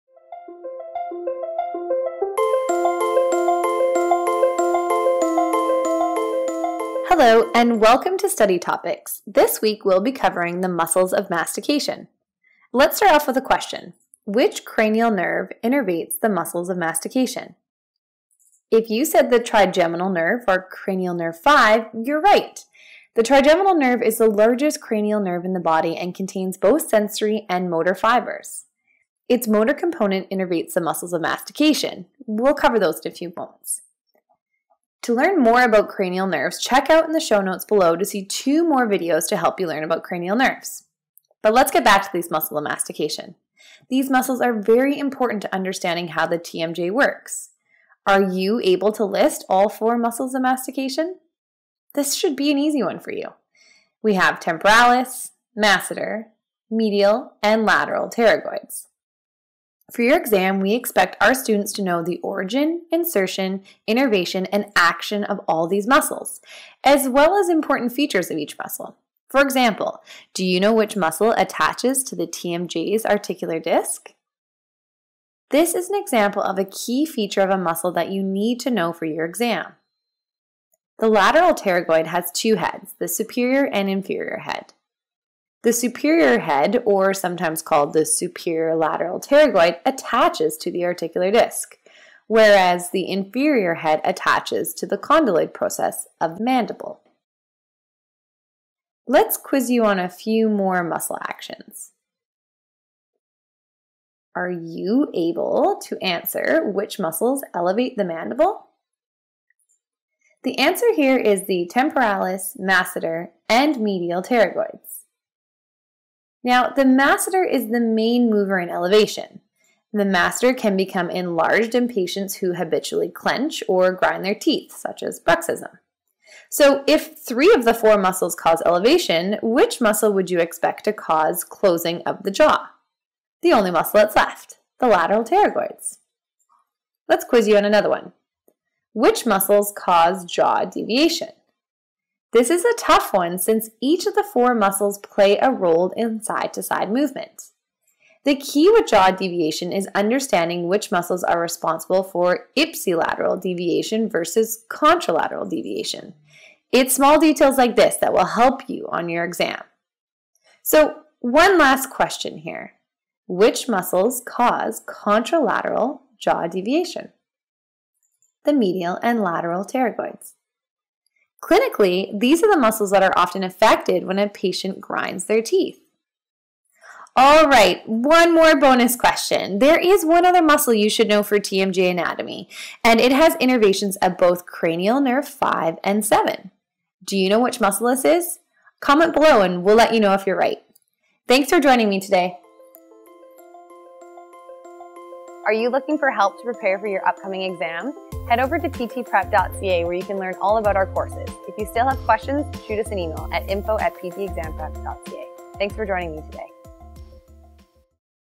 Study Topics: Muscles of Mastication
This week in Study Topics, we're reviewing the four muscles involved in mastication.